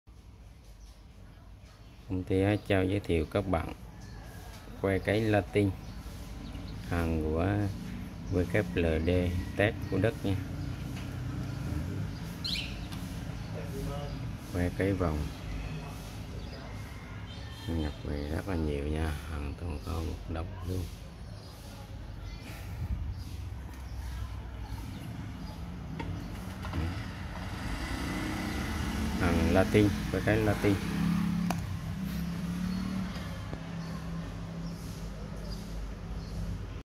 Que cấy Platinum / Iridium WLD-Tec Đức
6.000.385 Platinum / Iridium Inoculation Loop with loop int 5mm , wire 0.6mm
hãng sản xuất : WLD-TEC / Đức
nhập khẩu : Công ty TNHH Thiết Bị Thí Nghiệm Á Châu